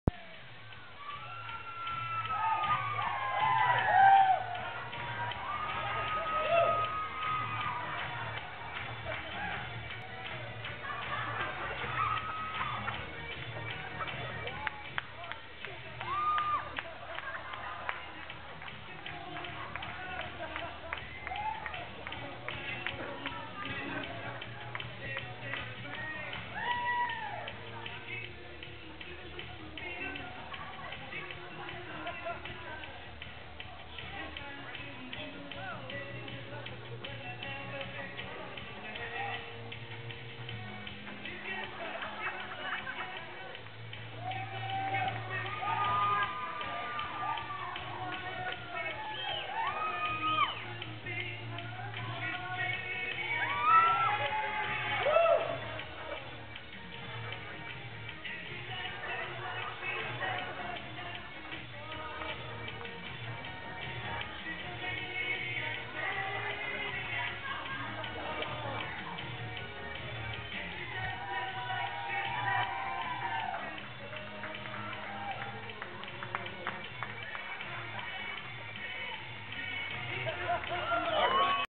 Jen and Tyler's Wedding Dance
Choreographed with a bit of improv!  Amazing!!!  :)